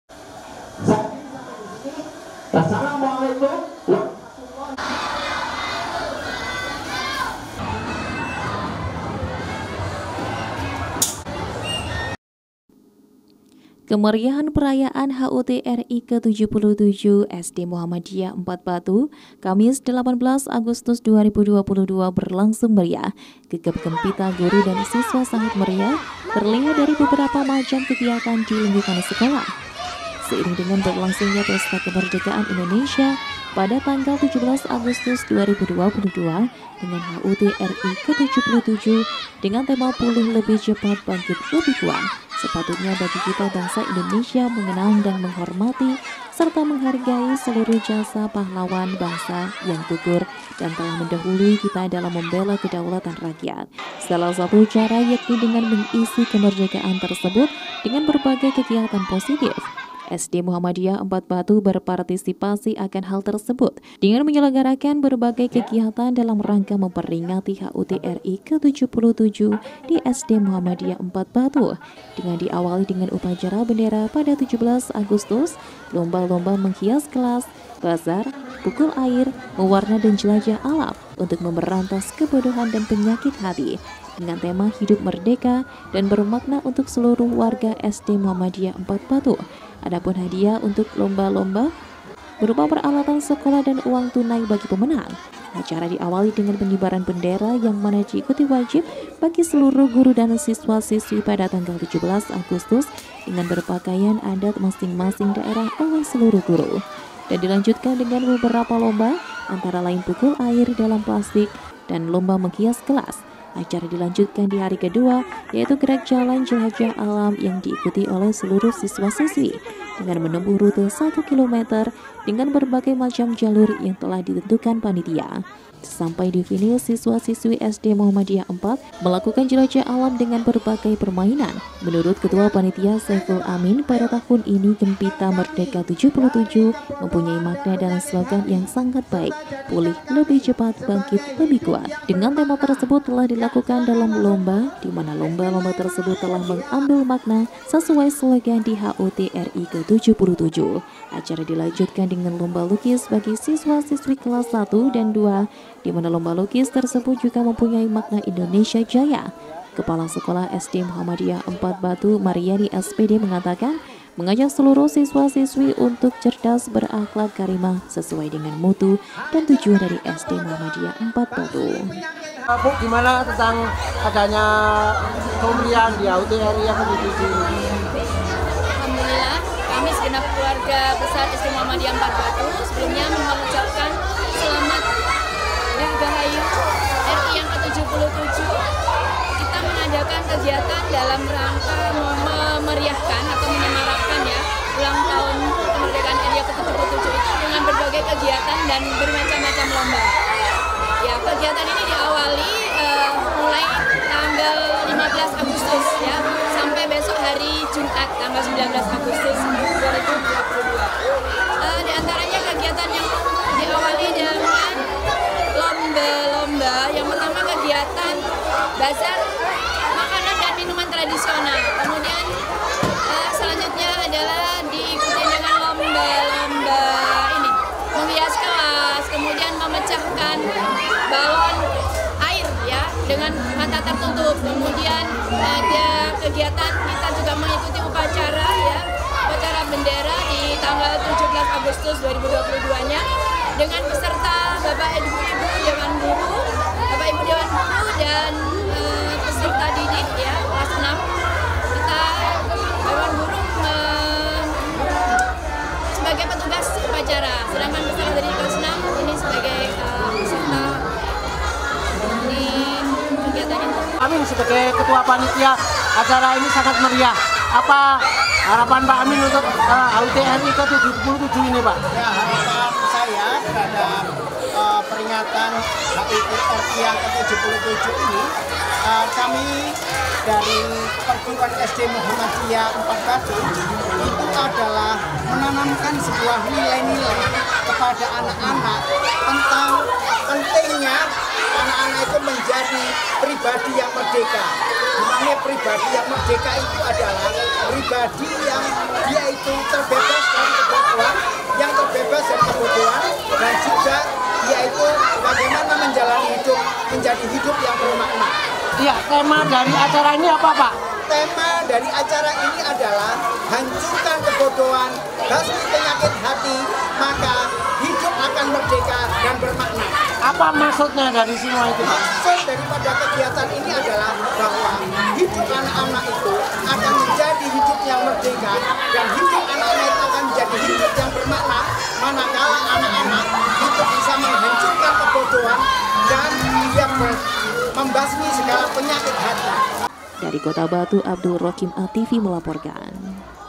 GEGAP GEMPITA HUT RI 77 SD MUHAMMADIYAH 4 BATU | ATV MALANG RAYA
KEMERIAHAN PERAYAAN HUT RI- 77 SD MUHAMMADIYAH 4 BATU. KAMIS 18 AGUSTUS 2022  BERLANGSUNG MERIAH, GEGAP GEMPITA GURU DAN SISWA SANGAT MERIAH TERLIHAT DARI BEBERAPA MACAM KEGIATAN DI LINGKUNGAN SEKOLAH.